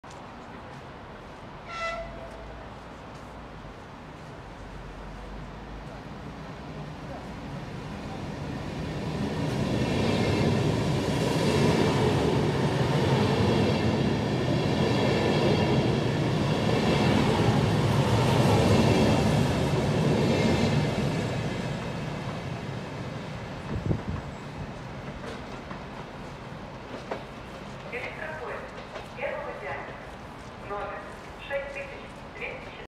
Опытный ЭШ2-001 Аэроэкспресс На станции Лианозово
ОПИСАНИЕ

Эксплуатация:
С завода поступил в собственность ООО «Аэроэкспресс»

09.2015 – участник в V Международного железнодорожного салона «ЭКСПО 1520» на территории Экспериментального кольца ВНИИЖТ

Вагоны и сетевые коды:
01 — 13178231
02 — 13262803
03 — 13505359
04 — 13262811
05 — 13505367
09 — 13178248